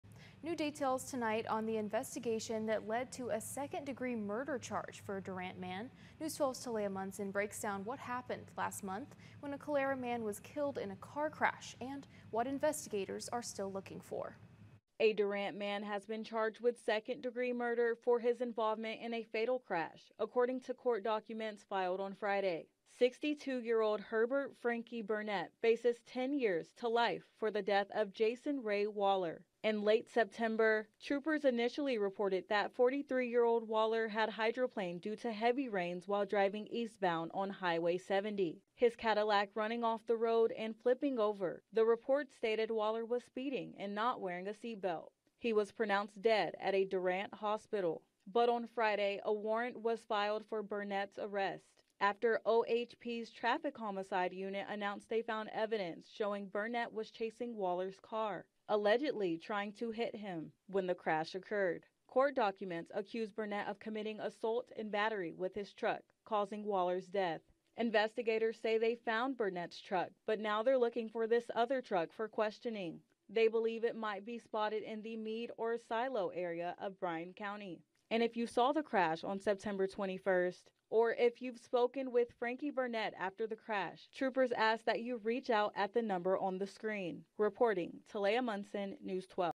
Durant man arrested for murder in fatal road rage crash
A Durant man has been charged with murder after a crash killed a man in Bryan County.